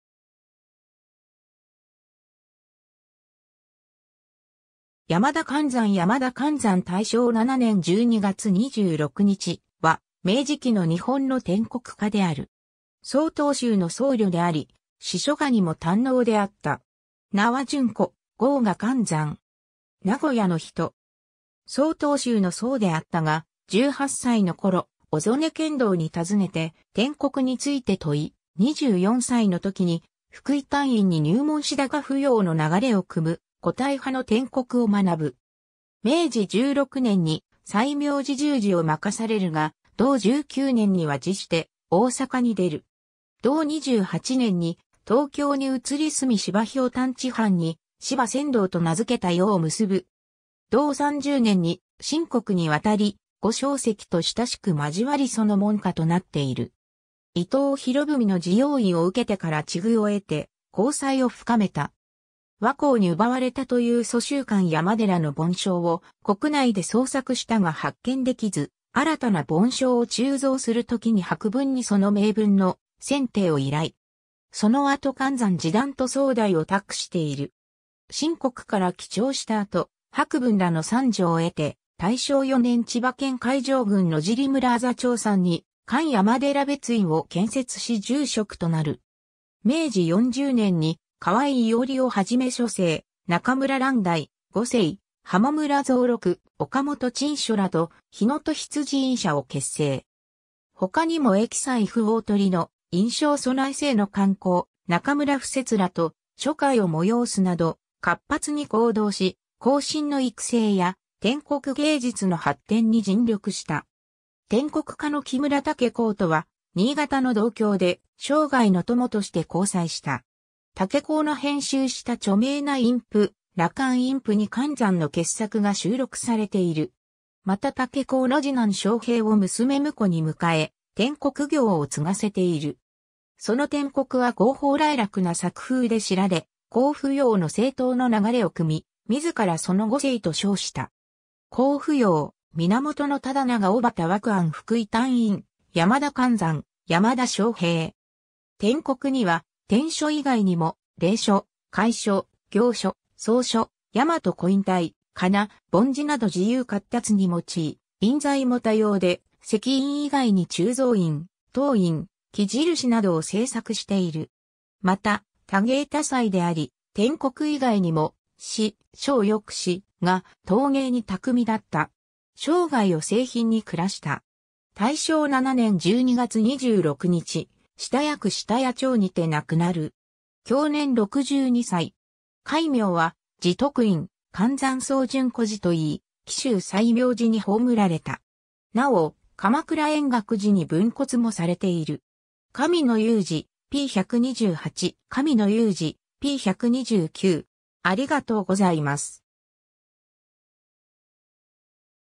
山田寒山
日本の禅僧_(曹洞宗)
山田寒山 山田 寒山（やまだ かんざん、男性、安政3年（1856年） – 大正7年12月26日（1918年））は、明治期の日本の篆刻家である。
曹洞宗の僧侶であり詩書画にも堪能であった。
 名は潤子、号が寒山。
名古屋の人。
 曹洞宗の僧であったが、18歳の頃、小曽根乾堂に訪ねて篆刻について問い、24歳のときに福井端隠に入門し高芙蓉の流れを汲む古体派の篆刻を学ぶ。
明治16年に最明寺（熊野市二木島町）住持を任されるが同19年には辞して大坂に出る。
同28年に東京に移り住み芝瓢箪池畔に芝仙堂と名付けた庵を結ぶ。
同30年に清国に渡り、呉昌碩と親しく交わりその門下となっている。
 伊藤博文の自用印を受けてから知遇を得て交際を深めた。
倭寇に奪われたという蘇州寒山寺の梵鐘を国内で捜索したが発見できず、新たな梵鐘を鋳造するときに博文にその銘文の撰定を依頼。
その後寒山寺檀徒総代を託している。
清国から帰朝した後、博文らの賛助を得て、大正4年（1915年）千葉県海上郡野尻村字長山（現在の銚子市長山町）に寒山寺別院を建設し住職となる。
 明治40年（1907年）に河井荃廬をはじめ初世中村蘭台・五世浜村蔵六・岡本椿所らと丁未印社を結成。
他にも益斎富鴻の『印章備正』の刊行、中村不折らと書会を催すなど活発に行動し、後進の育成や篆刻芸術の発展に尽力した。
 篆刻家の木村竹香とは新潟の同郷で生涯の友として交際した。
竹香の編集した著名な印譜『羅漢印譜』に寒山の傑作が収録されている。
また竹香の次男正平を娘婿に迎え、篆刻業を継がせている。
 その篆刻は豪放磊落な作風で知られ、高芙蓉の正統の流れを汲み、自らその五世と称した。
 高芙蓉 – 源惟良– 小俣蠖庵 - 福井端隠 – 山田寒山 - 山田正平
 篆刻には篆書以外にも隷書・楷書・行書・草書・大和古印体・仮名・梵字など自由闊達に用い、印材も多様で石印以外に鋳造印・陶印・木印などを製作している。
また多芸多才であり、篆刻以外にも詩・書をよくし、画・陶芸に巧みだった。
生涯を清貧に暮らした。
 大正7年(1918年)12月26日、下谷区下谷町にて亡くなる。
享年62歳。
戒名は「自得院寒山想潤居士」といい、紀州最明寺に葬られた。
なお、鎌倉円覚寺に分骨もされている。
 ^ 神野雄二、p.128. ^ 神野雄二、p.129.